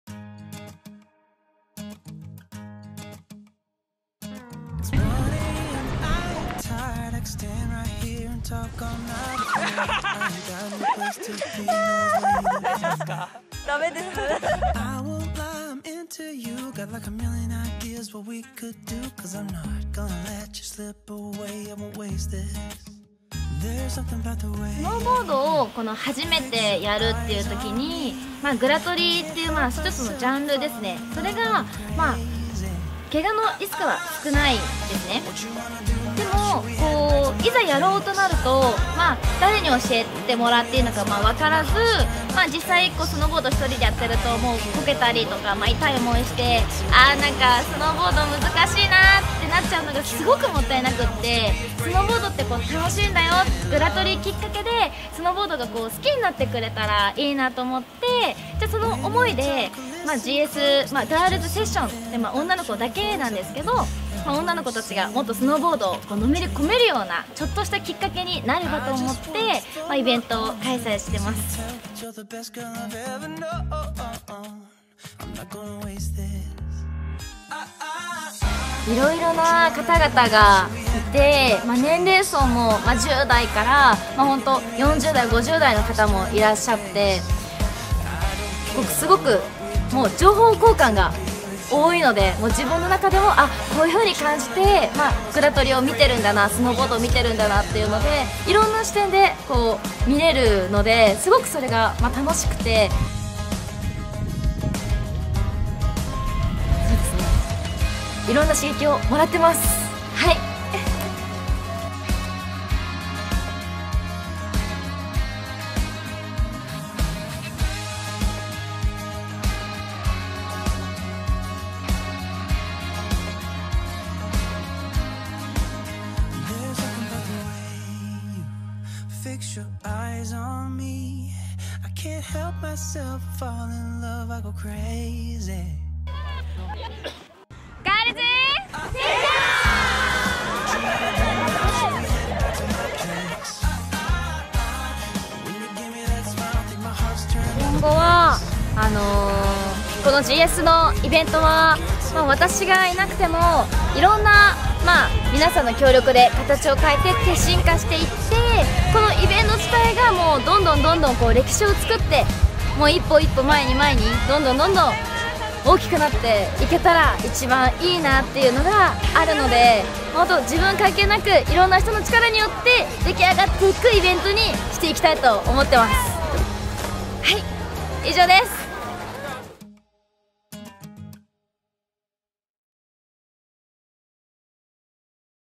G×S 2020版 ｸﾞﾗﾄﾘｶﾞｰﾙｽﾞｾｯｼｮﾝ
2020年 G×S のハイライト動画です。
今年24-25のスノーボードシーズンでも盛り上がって行きましょう！